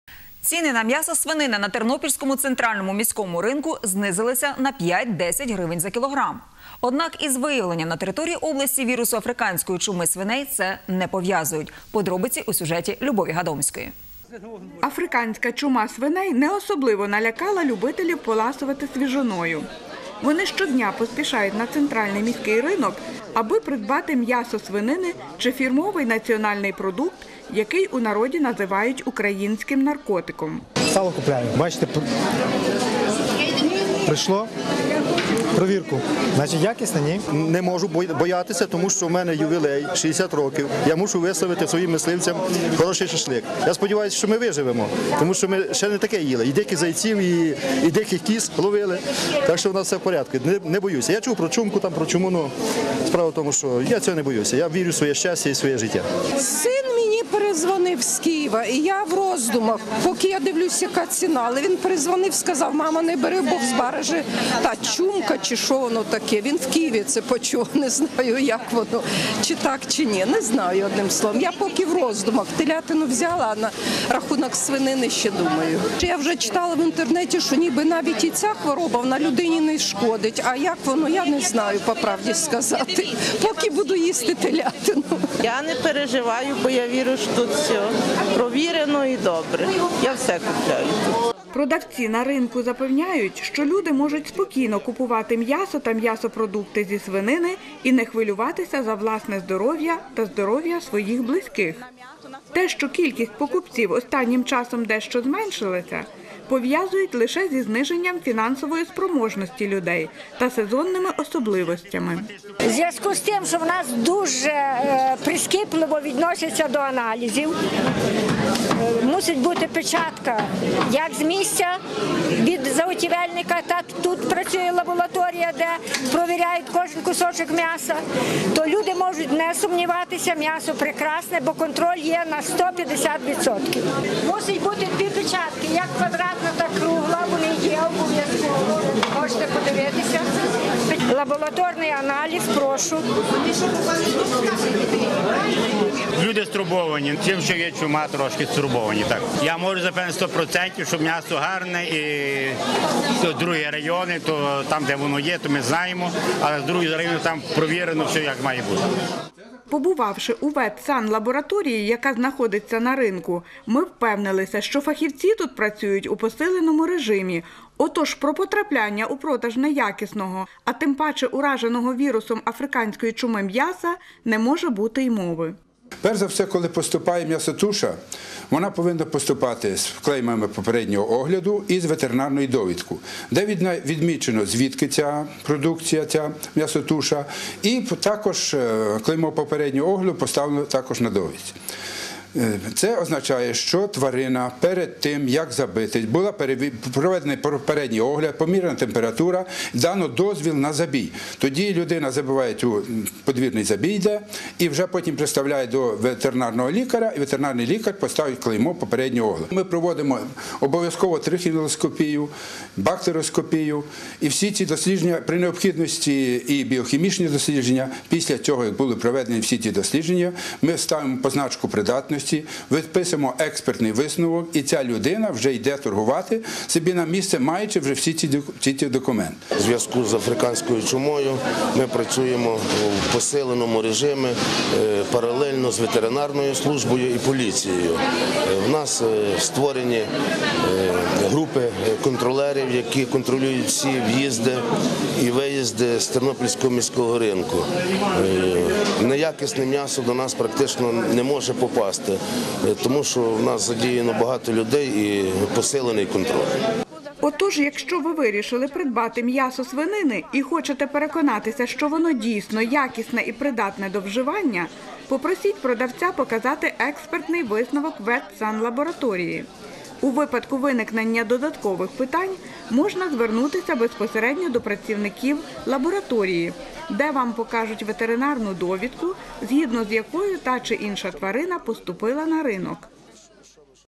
Африканська чума свиней не є причиною зниження цін на м'ясо в Тернополі
Ціни на м'ясо свинини та яловични на Тернопільському центральному  міському ринку знизилися на 5-10 гривень за кілограм. Однак із виявленням на території області вірусу африканської чуми свиней це не пов"язують.